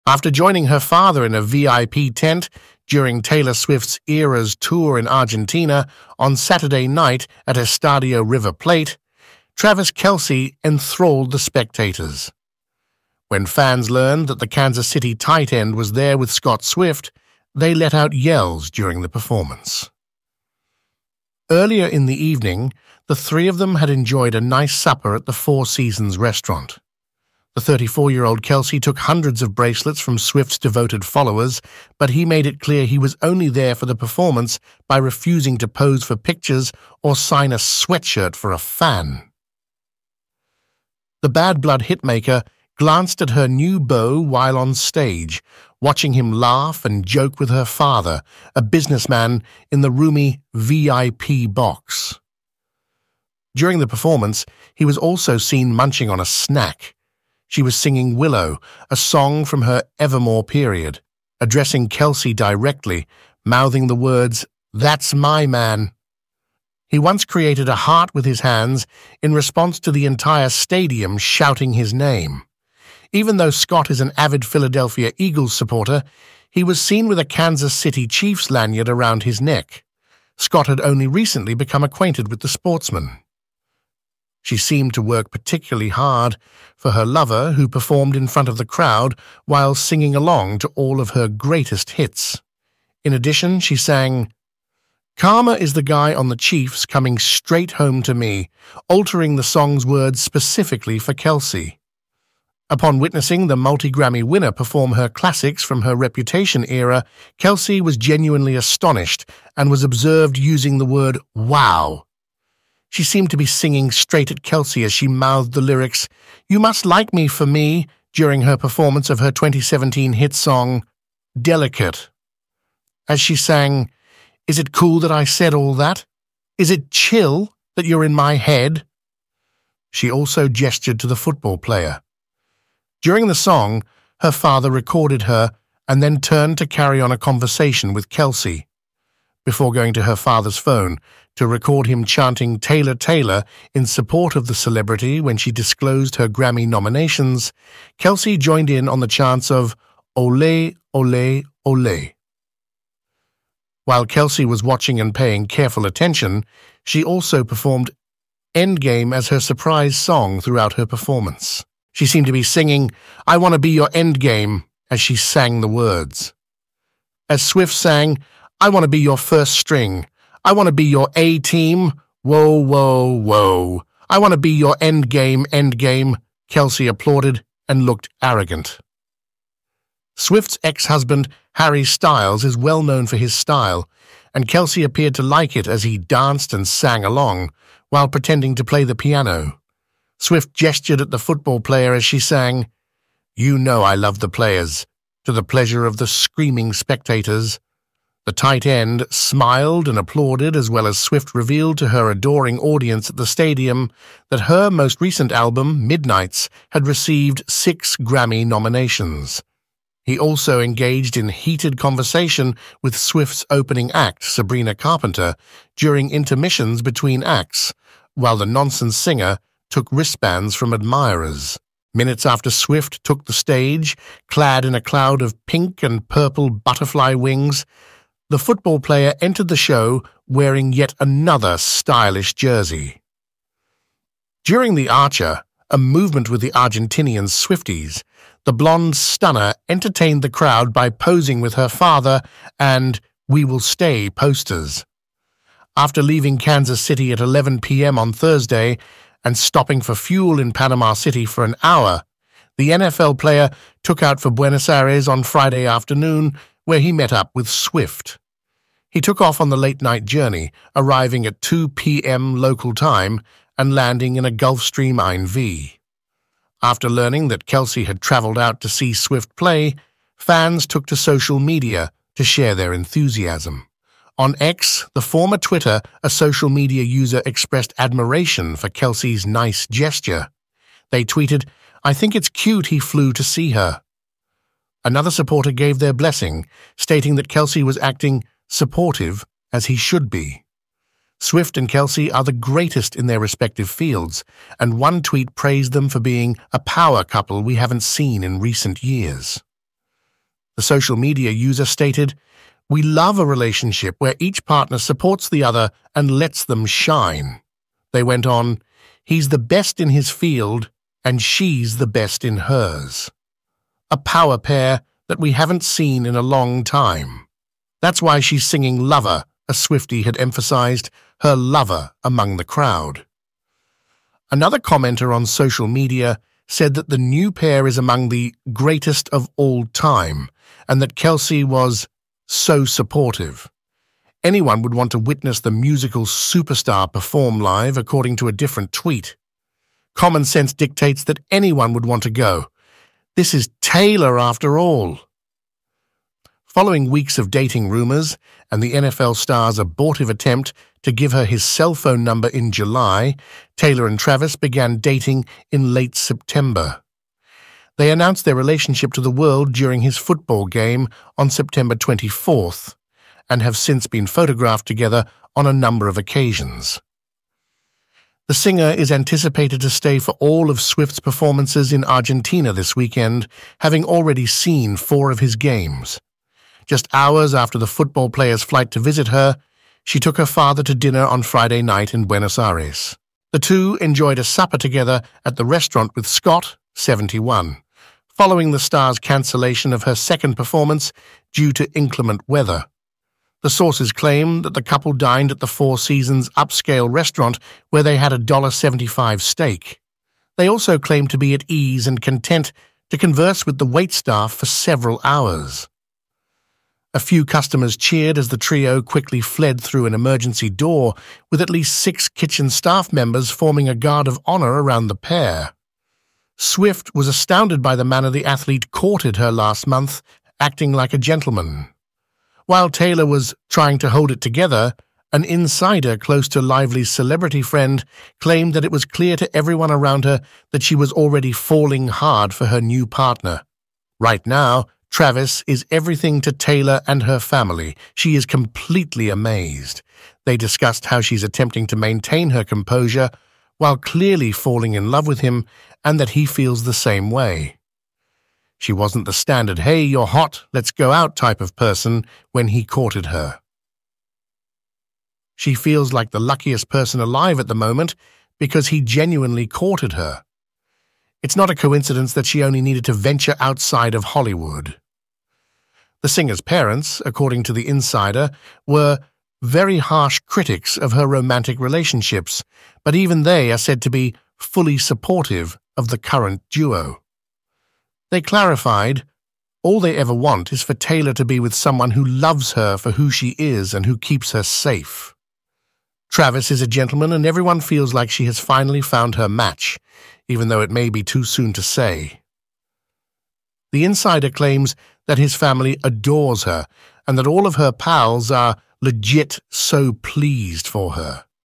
Travis Kelce: Taylor Swift’s #1 Fan? | Eras Tour Argentina
In this video, you will see the exclusive footage of Taylor Swift and Travis Kelce at the Eras Tour in Argentina. You will witness their amazing chemistry, their sweet moments, and their epic performance. You will also learn about their relationship status, their Grammy nominations, and their interactions with other celebrities. This is a video you don’t want to miss!